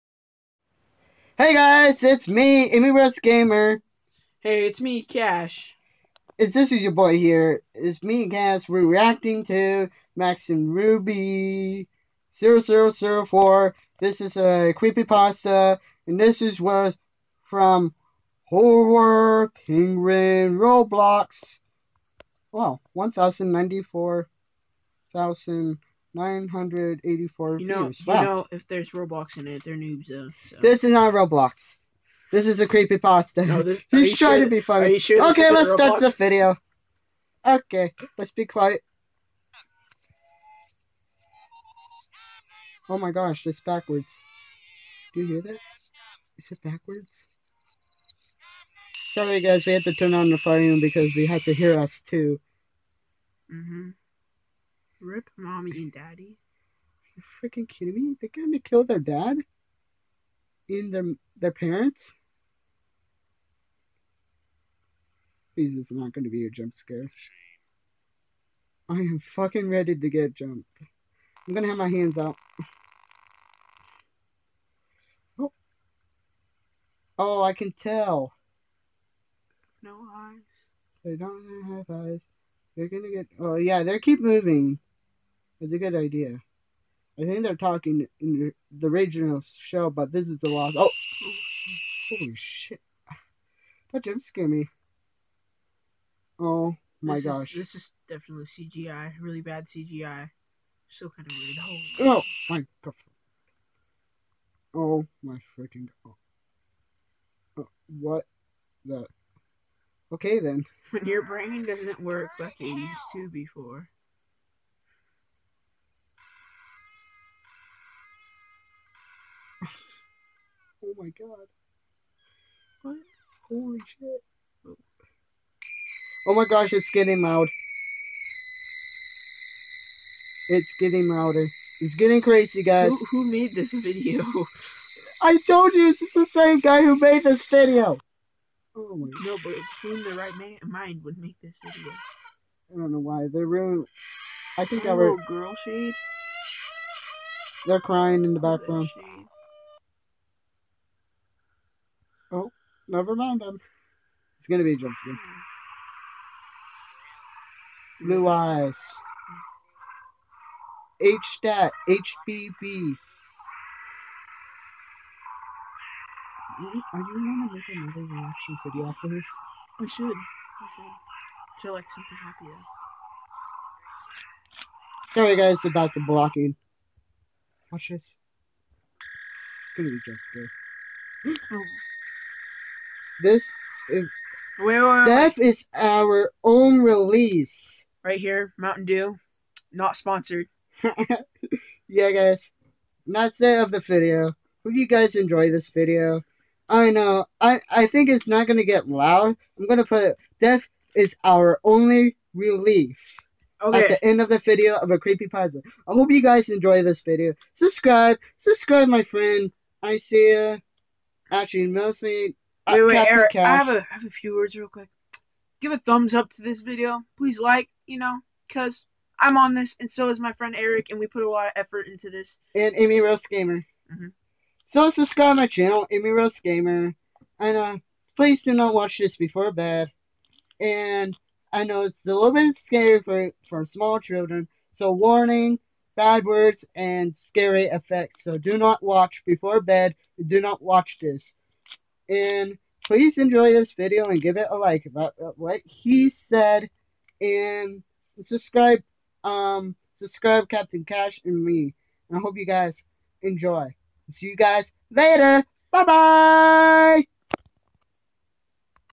Reacting friday l max and ruby 0004 l death is our only relese
Warning bad words and scary effects me and Captain cash react to max and ruby 0004 creepypasta so enjoy this video.